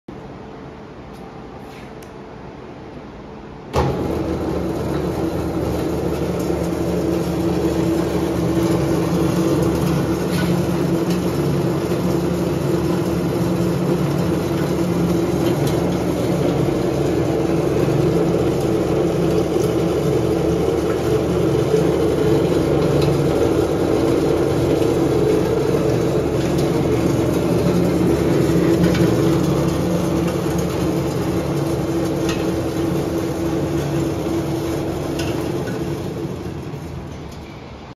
6" x 8" Conair Martin Granulator, Model CR69
Used 6" x 8" Conair Martin Granulator, Model CR69, 3 HP, New In 1995

Staggered Style Rotor,1/4" Screen Size, 2" Diameter Discharge, Serial #250.734

Machine Dimensions:
38" x 32" x 71"